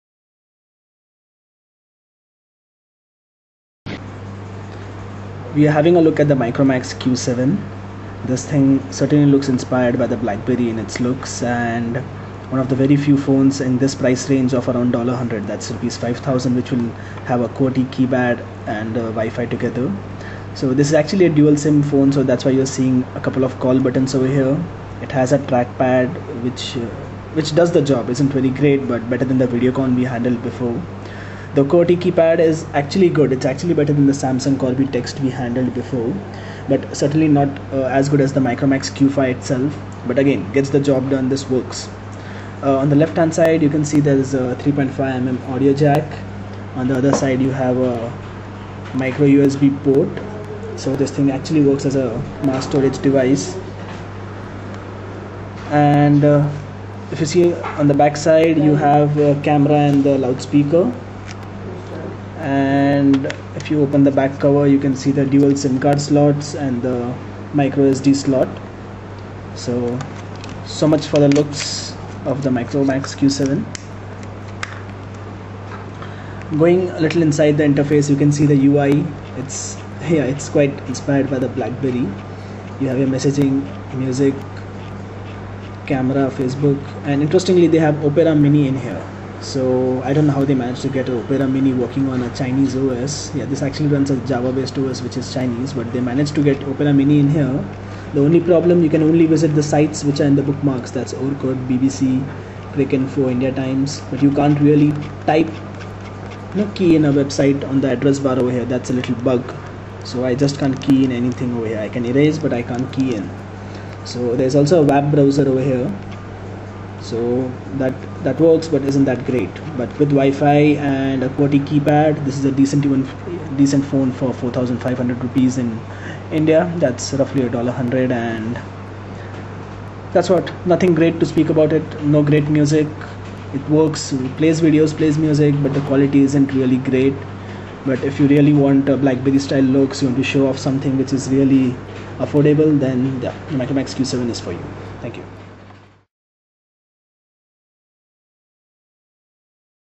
Reviewing the Micromax Q7 mobile
Guess I got a little confused on the USD conversation rate on the video, Corrected as 100$ = Rs 4500! The Opera Mini issue also gets resolved if you install the java file for Opera Mini 5 on this. review